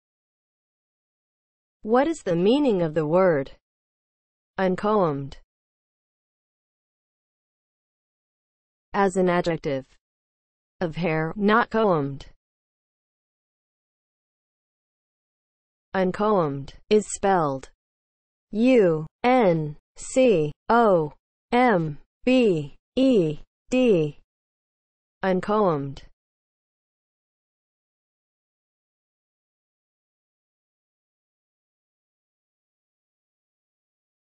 What is the meaning of the word UNCOMBED?
Definition and spelling of the word UNCOMBED.

(00:00) Intro
(00:08) As an adjective
(00:15) Spelling

As an adjective (00:08):
• (of hair) not combed